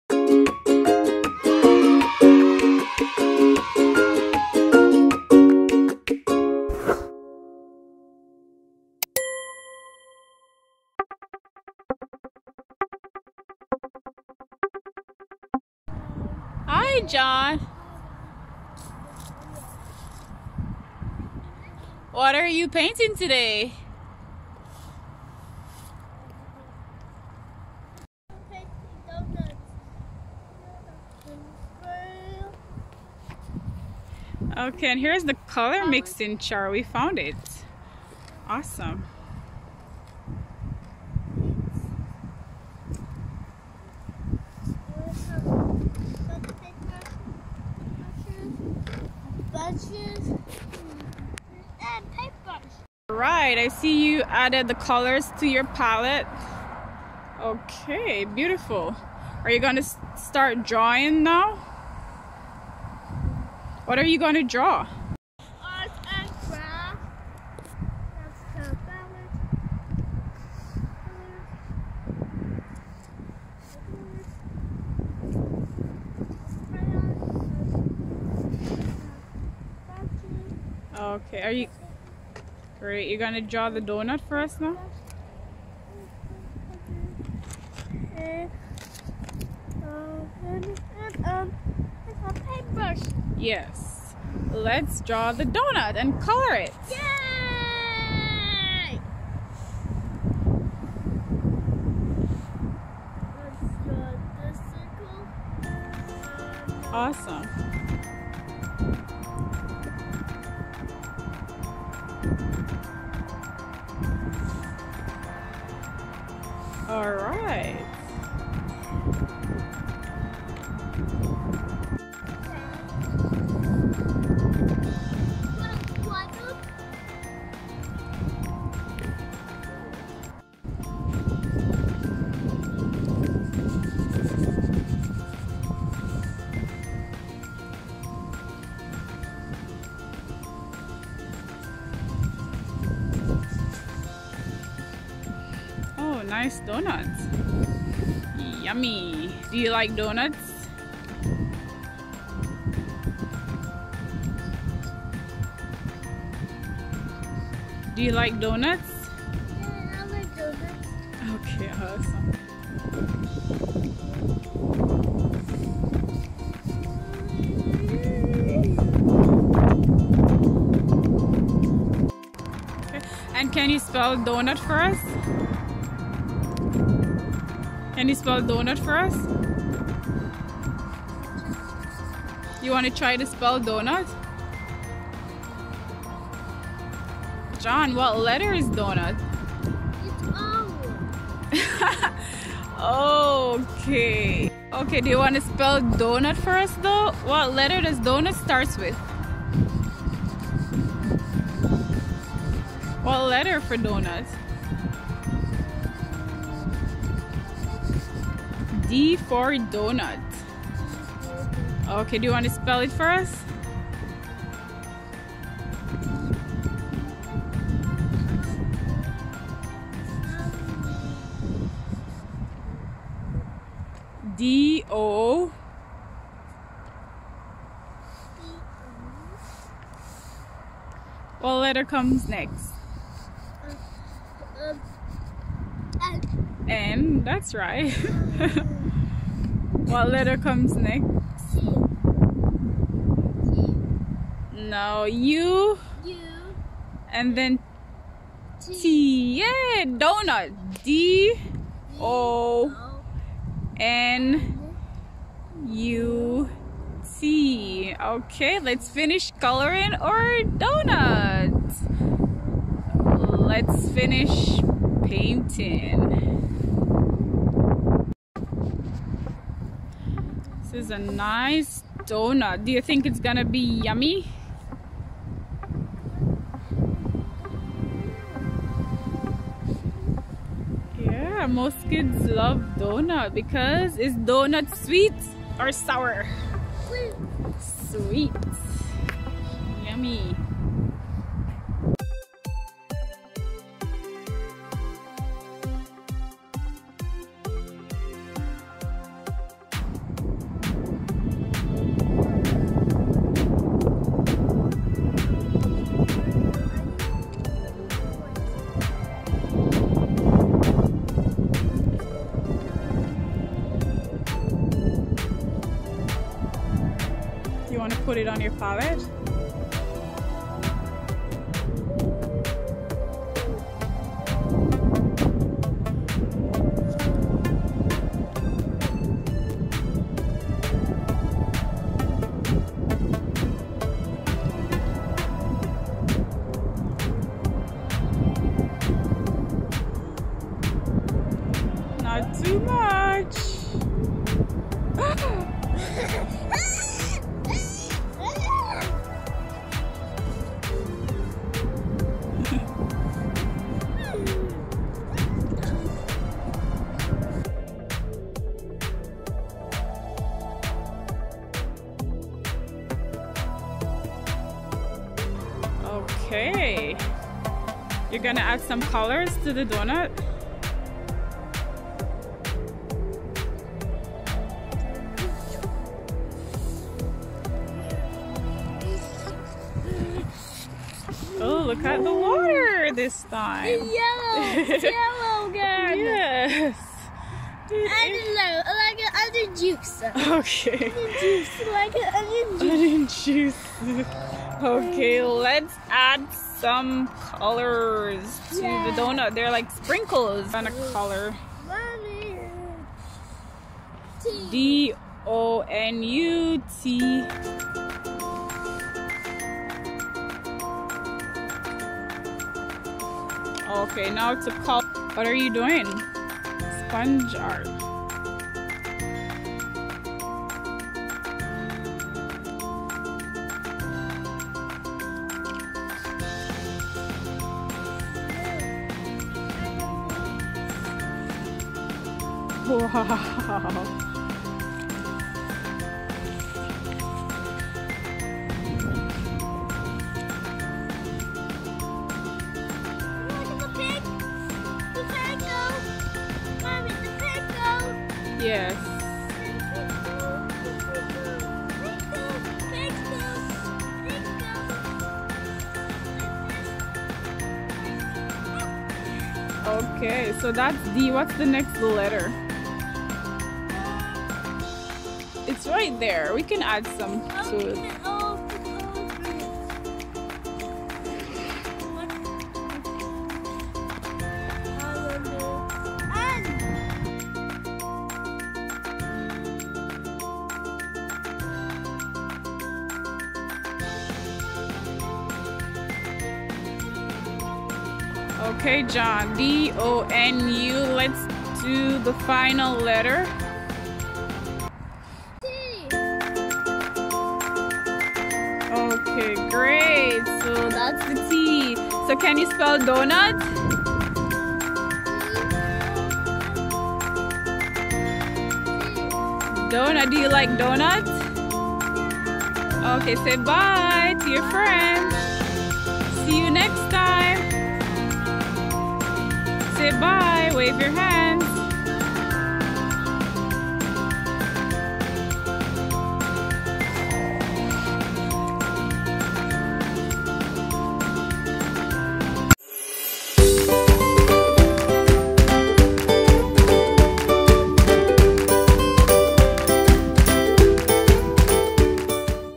Art Fun - Donut
It's time for Art Fun in the Park. Let's get creative with some art and craft.